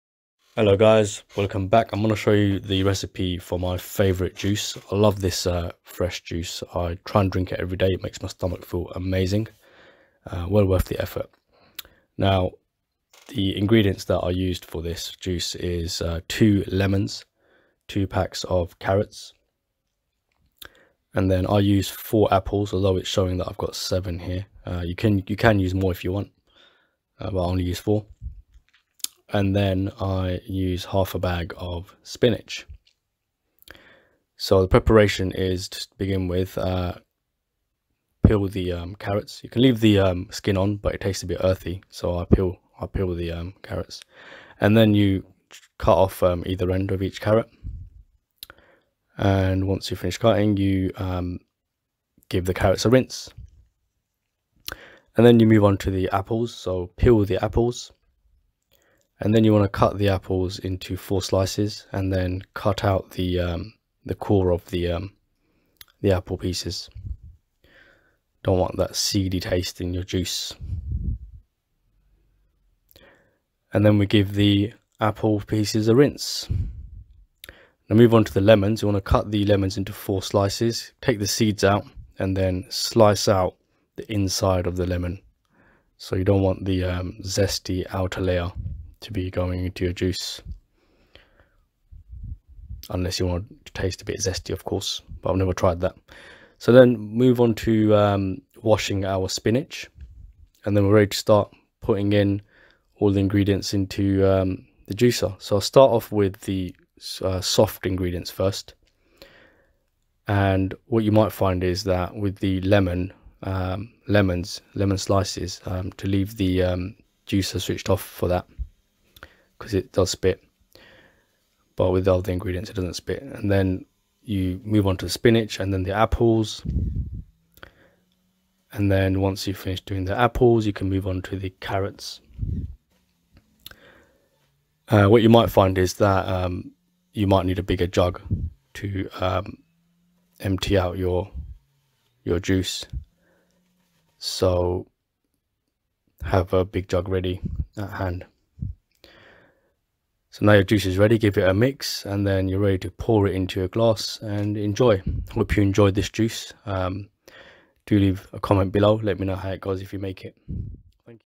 My favourite fresh juice
Ingredients:

Lemons
Carrots
Apples
Spinach

Let me know if you try it!